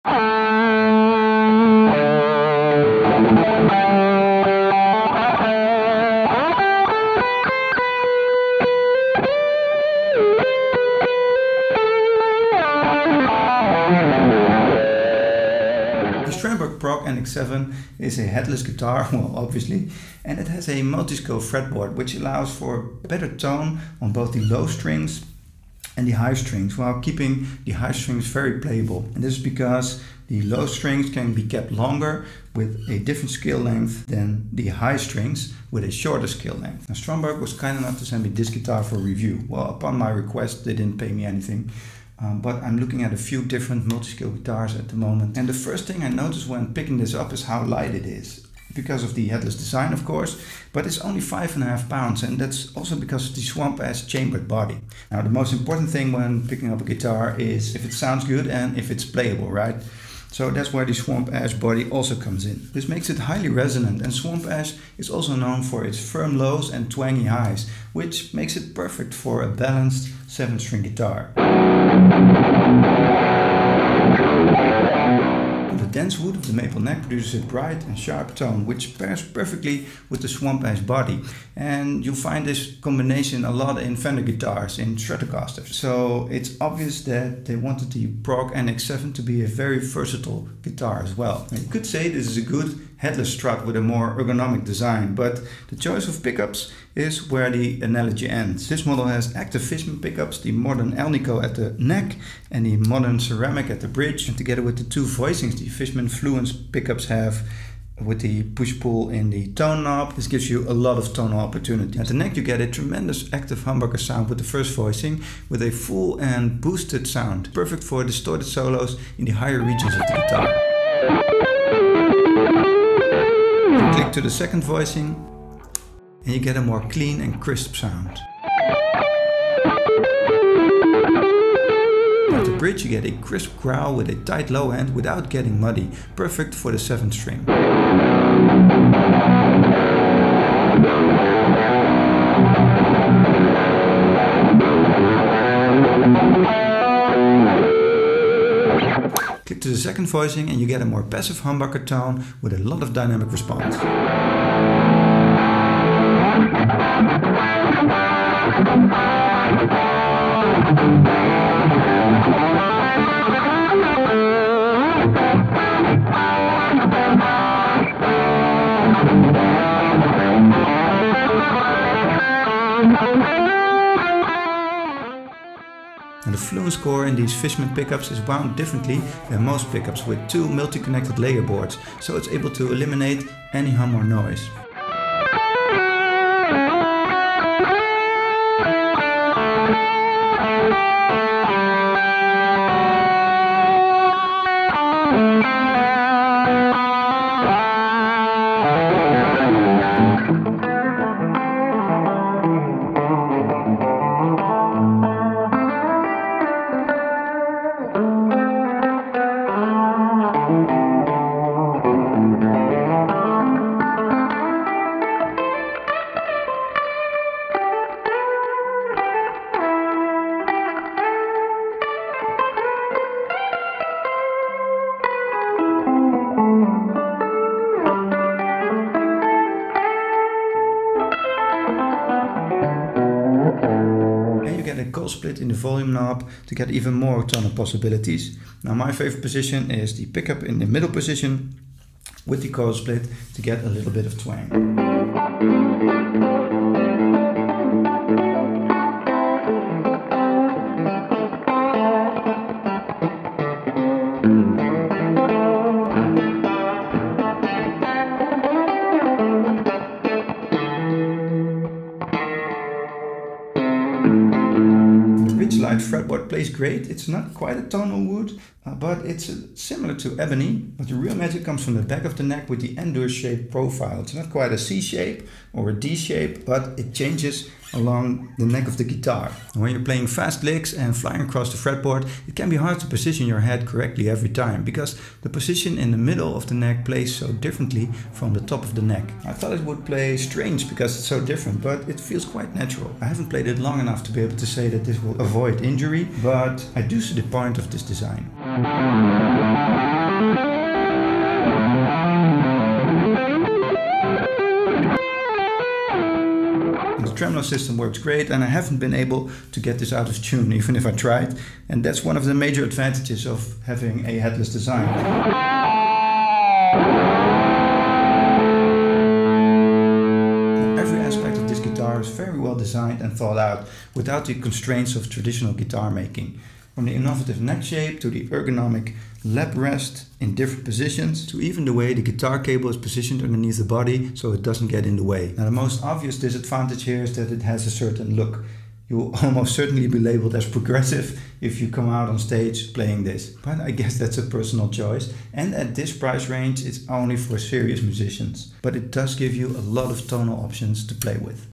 Going Headless: Strandberg Boden Prog NX7 Review
In this video, I review the Strandberg Boden Prog NX7. A headless guitar is a favorite for many guitarists. Well, not that many, actually. It's kind of a niche thing.

Probably because it looks so different, many players aren't yet used to the idea. But since it's lighter, it's a lot easier to hold, and the weight distribution is perfect. This one has a fanned fret multiscale fretboard.